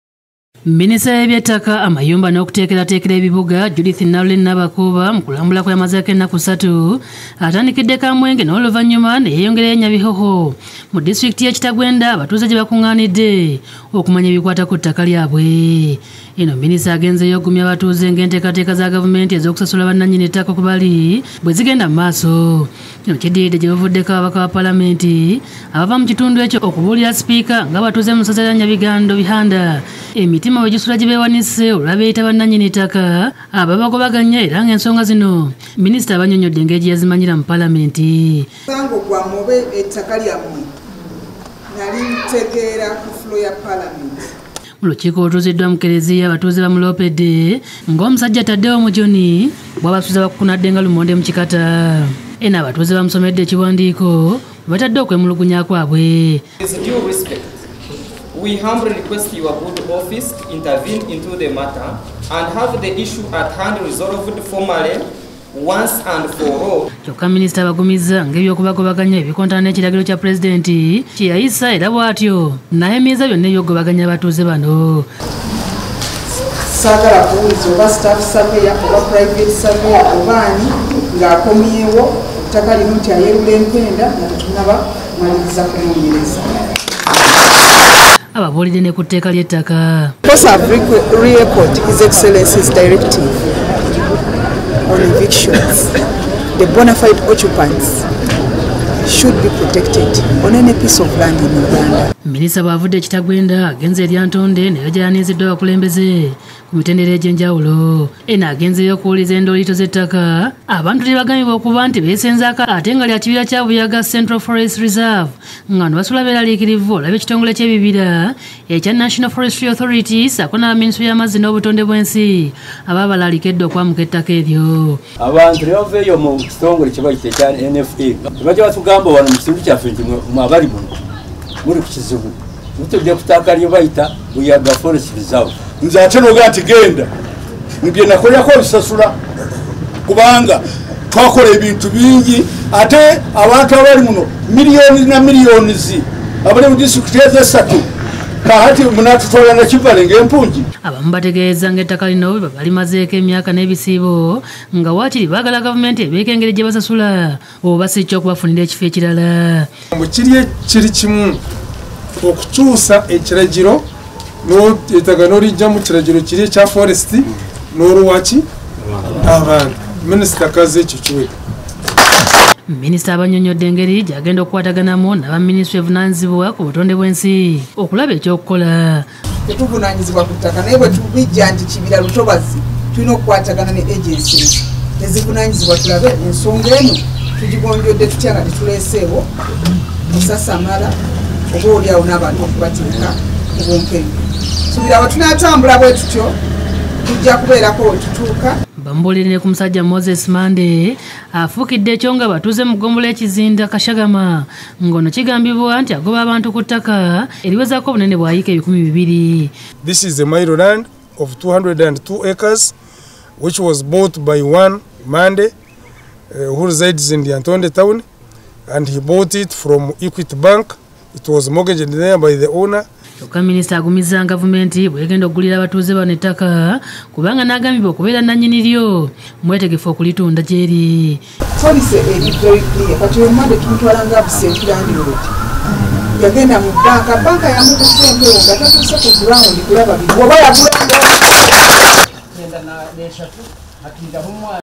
Okutaasa abagobwa ku Ttaka, Minisita Nabakooba Agumizza Abatuuze
Gavumenti ya Uganda ng’eyita mu minisitule y’ebyettaka,Amayumba n’okukulaakulanya Ebibuga egumizza abatuuze mu Disitulikiti y’e Kitagwenda ne Lyantonde ng’enteekateeka ez’okugula ettaka kwebatudde bwezigenda mu maaso okusobola okubataasa ku Bannanyini lyo ababeewanisizza emitima.Bino bituukiddwako minisita Judith Nalule Nabakooba bw’abadde agenzeeyo mu kulambula kw’amazeeko ennaku ssatu.

Omusomi: Ismail Nsubuga
Omusasi: